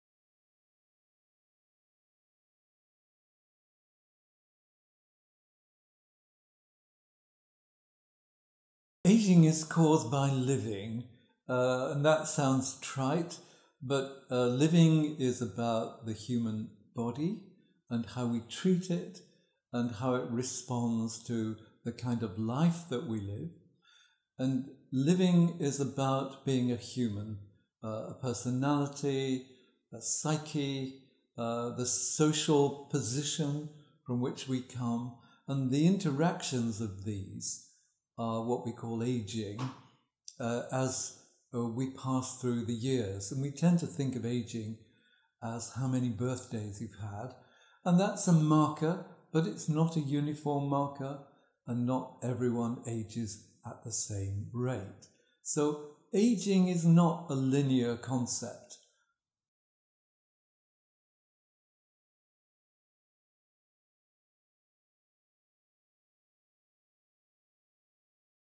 What causes ageing? Ageing Unlocked: with Professor Malcolm Johnson
Guild Living met with Malcolm Johnson, Professor of Gerontology and End of Life at University of Bath, as part of our Ageing Unlocked series. In this instalment, we talk about the process of ageing, how to slow it down, and how it impacts lifestyle in later life.

As a professor of Health and Social Policy at the University of Bath since 1995, Professor Johnson has written twelve books, including ‘Spiritual Dimensions of Ageing’, and over 160 chapters, articles and monographs. The Professor has recently attracted the public’s attention as the lead experimenter on Channel 4’s ‘Old People’s Home for 4 Year Olds’, which sees pre-school children sharing their classroom with older people in a Bristol retirement home.

The professor is now working with Guild Living to create an academic-led approach to support better ageing in later life.

What causes ageing?

Ageing is caused by living. Living is about the human body, and how we treat it and how it responds to the kind of life that we live. Living is about being human, a personality, a psyche, the social position from which we come and the interactions of these are what we call ageing. As we pass through the years, we tend to think of ageing as how many birthdays we’ve had and that’s a marker but not a uniform marker – and not everyone ages at the same rate. Ageing is not a linear concept.

How can you live to be ‘very old’?

Very often taxi drivers will say to me, what do you do? And I’ll say something like, “I’m an academic” and eventually I’ll say, “Well, I am a professor and I study ageing”. And then they will ask, “How do you live to be very old?”. If I am really tired, I say: “Choose your parents carefully”. Your genetic inheritance is clearly important. That is not the only factor or the only variable, but you can be lucky and get good genes and that could help you live for a long time.

If your parents live to be really old then, unless you are very unfortunate or you wreck your body by doing bad things to it or bad things happen to you, then you too are likely to live to be as old as your parents. Quite likely older, as each succeeding generation is living longer. So, the taxi drivers sometimes get the very short answer and they go away and think about it and worry about their mum and dad – or sometimes we have a longer conversation about lifestyle and other variables.

Is there a link between education and ageing?

Sometimes we talk about the fact that there is strong evidence that the more education you have, had the longer you will live. This is not personal, but those who have studied for a long time, and have more degrees than other people and pursued an academic life, can continue after other people may stop and on the whole live longer.

That is partly due to the joy of a challenging occupation, but also the capacity to manage your own life – which is what we call ‘agency’. The opportunity to make your own decisions and organise your life in a way that suits you and allows you to go on maintaining that lifestyle of freedom and opportunity long into the later years of your life.

What about a link between lifestyle and ageing?

During my early career, I discovered that the oldest man in Wales was a coal miner and – according to academic evidence – most coal miners died before they were 60. He lived for more than a 100 years and, although he went down a mine each day and ingested all manner of things into his body, he didn’t drink alcohol and he didn’t smoke cigarettes. One reason he lived so long was because he didn’t drink anything other than hot water. Though, I do wonder if life was worth living that long just to drink hot water!

To read more from Professor Johnson on ageing, and its impact on lifestyle and society, make sure you follow our Ageing Unlocked series.